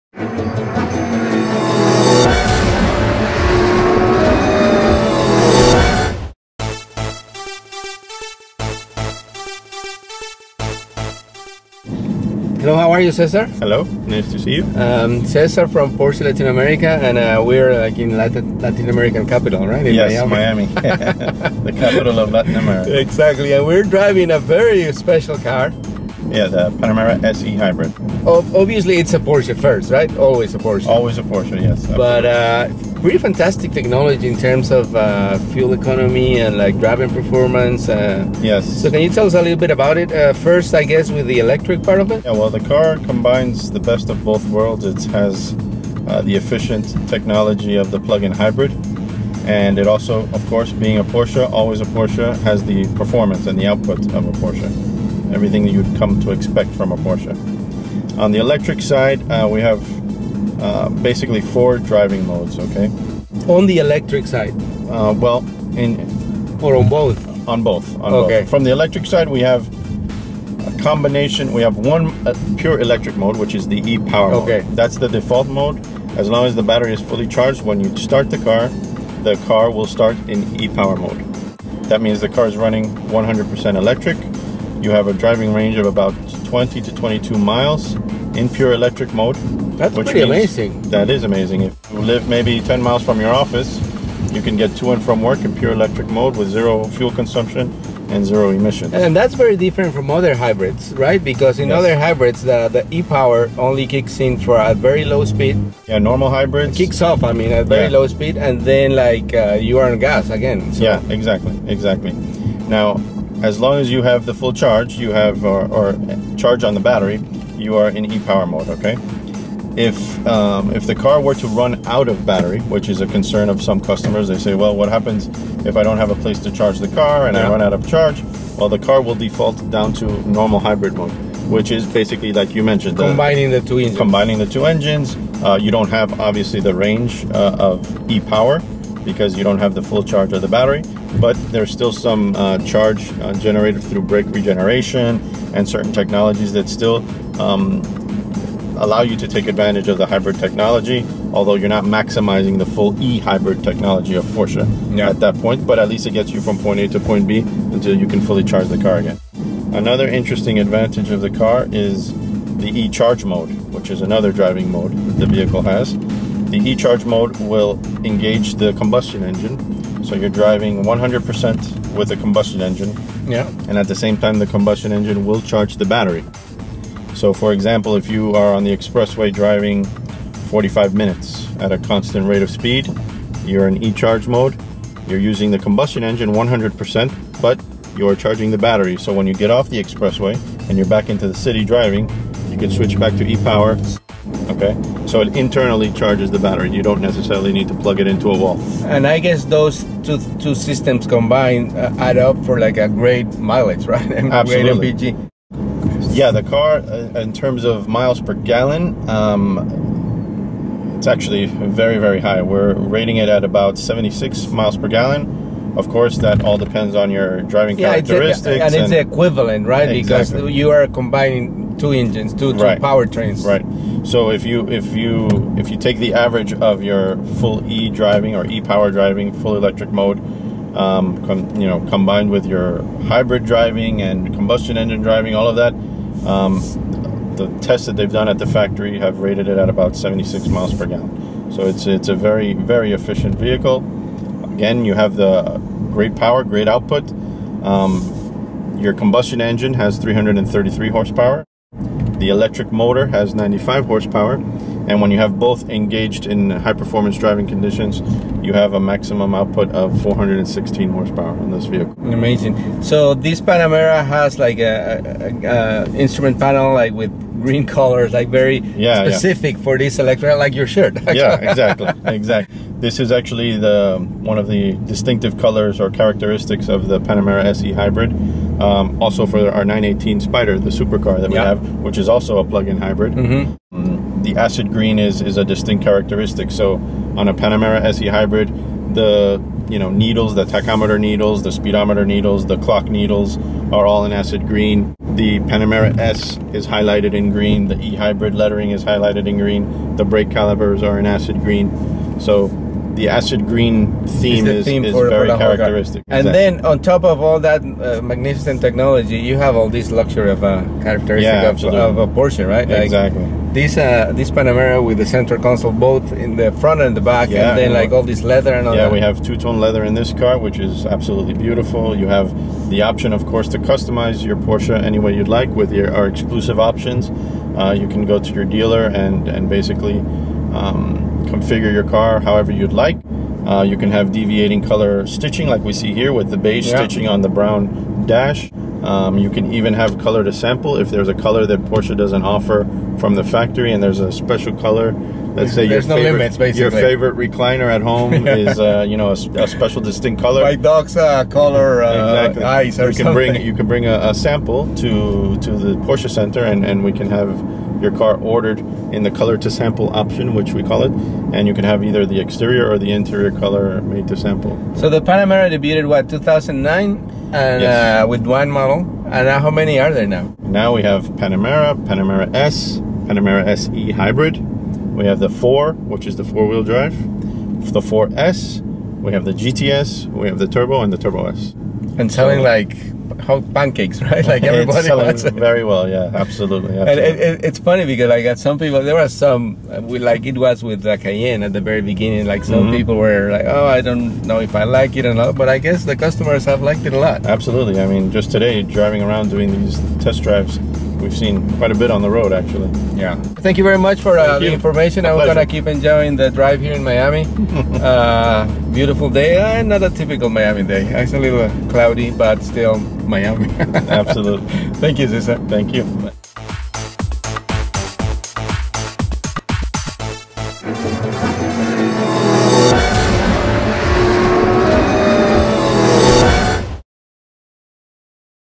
2014 Panamera S E-Hybrid Test Drive Miami
The new Panamera S E-Hybrid improves upon the concept offered by the previous Panamera S Hybrid with a more powerful electric motor, a higher-performance battery that supplies more energy and is able be recharged from home or public vehicle charging stations.

The electric drive produces 95 horsepower, more than double the power of the previous modelâ€™s electric motor (47 hp). Electricity is stored in a newly developed lithium-ion battery, which at 9.4 kWh has increased capacity over the previous battery (1.7 kWh).

Via the integrated on-board charging componentry and the standard Porsche Universal Charger (AC), the battery can reach full charge within approximately two and a half hours when connected to a 240V power source.

The Panamera S E-hybrid is capable of accelerating from a standstill to 60 mph in 5.2 seconds on its way to a top track speed of 167 mph.